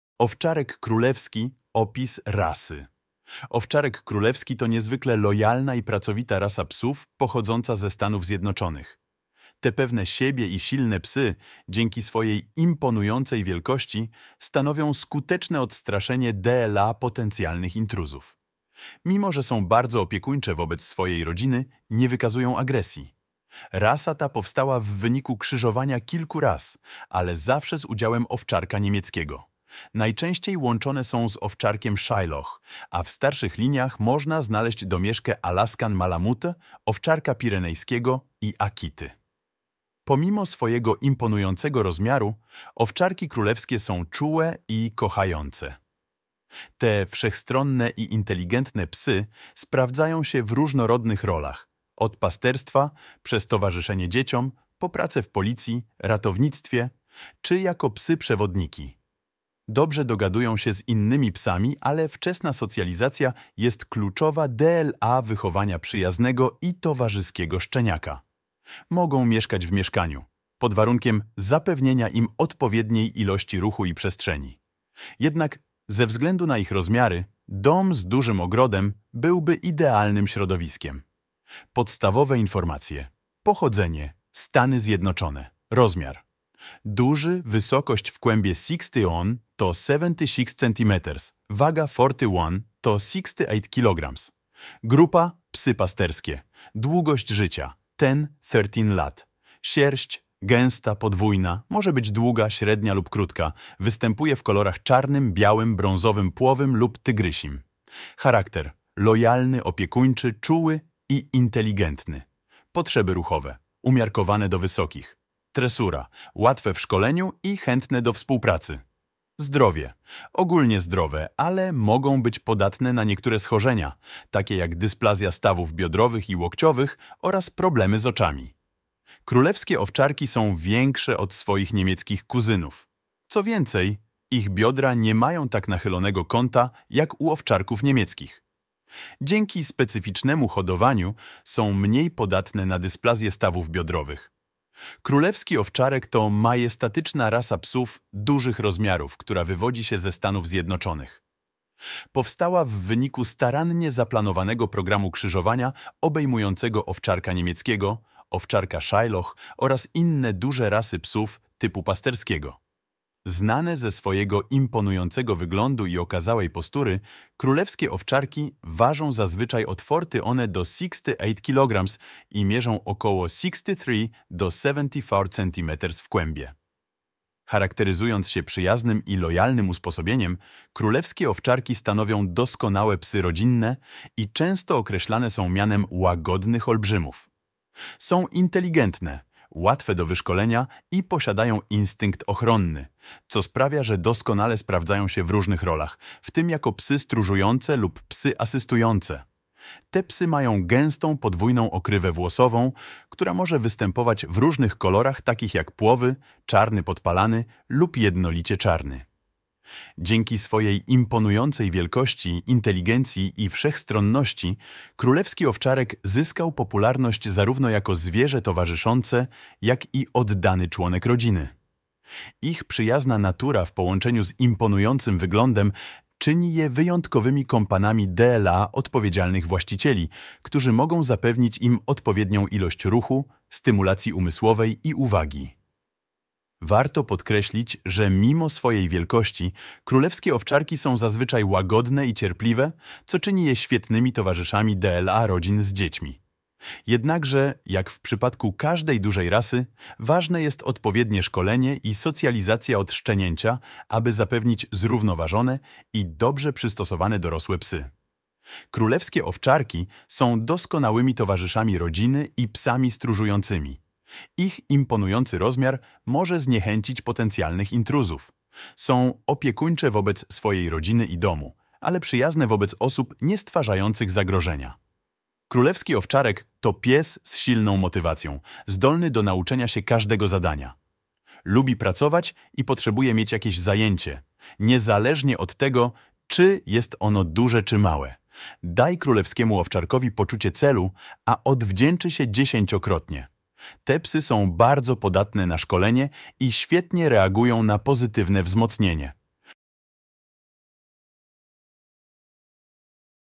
Owczarek królewski – Opis rasy
Królewski Owczarek to majestatyczna rasa psów dużych rozmiarów, która wywodzi się ze Stanów Zjednoczonych. Powstała w wyniku starannie zaplanowanego programu krzyżowania, obejmującego Owczarka Niemieckiego, Owczarka Shiloh oraz inne duże rasy psów typu pasterskiego. Znane ze swojego imponującego wyglądu i okazałej postury, Królewskie Owczarki ważą zazwyczaj od 41 do 68 kg i mierzą około 63 do 74 cm w kłębie.

Charakteryzując się przyjaznym i lojalnym usposobieniem, Królewskie Owczarki stanowią doskonałe psy rodzinne i często określane są mianem “łagodnych olbrzymów”. Są inteligentne, łatwe do wyszkolenia i posiadają instynkt ochronny, co sprawia, że doskonale sprawdzają się w różnych rolach, w tym jako psy stróżujące lub psy asystujące. Te psy mają gęstą, podwójną okrywę włosową, która może występować w różnych kolorach, takich jak płowy, czarny podpalany lub jednolicie czarny.

Dzięki swojej imponującej wielkości, inteligencji i wszechstronności, Królewski Owczarek zyskał popularność zarówno jako zwierzę towarzyszące, jak i oddany członek rodziny. Ich przyjazna natura w połączeniu z imponującym wyglądem czyni je wyjątkowymi kompanami dla odpowiedzialnych właścicieli, którzy mogą zapewnić im odpowiednią ilość ruchu, stymulacji umysłowej i uwagi.

Warto podkreślić, że mimo swojej wielkości, Królewskie Owczarki są zazwyczaj łagodne i cierpliwe, co czyni je świetnymi towarzyszami dla rodzin z dziećmi. Jednakże, jak w przypadku każdej dużej rasy, ważne jest odpowiednie szkolenie i socjalizacja od szczenięcia, aby zapewnić zrównoważone i dobrze przystosowane dorosłe psy.